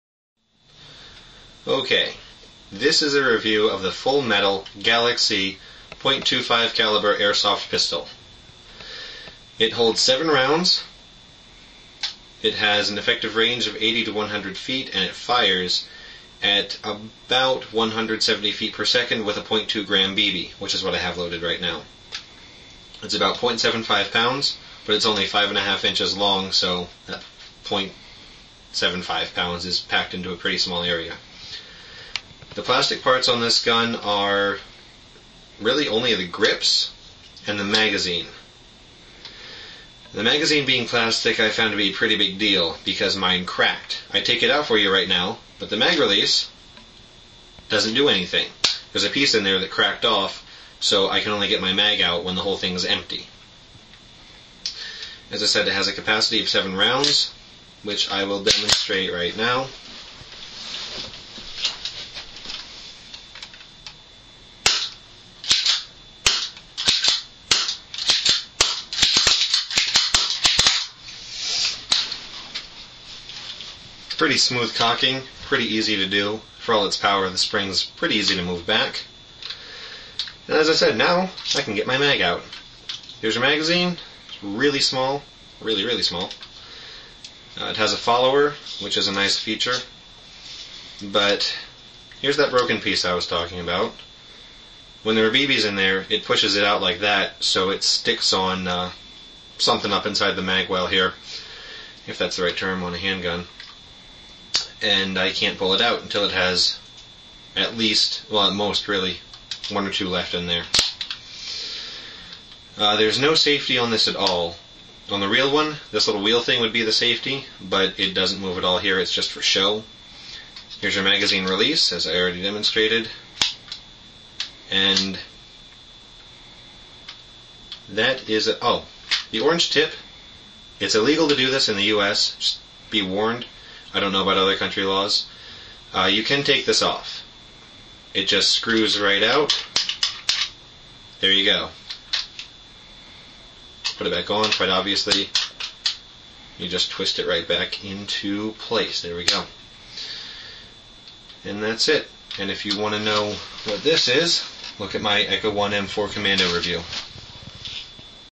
Galaxy Spring 25 Airsoft Pistol Review
This is a review on the full metal Galaxy 25 Airsoft Spring Pistol.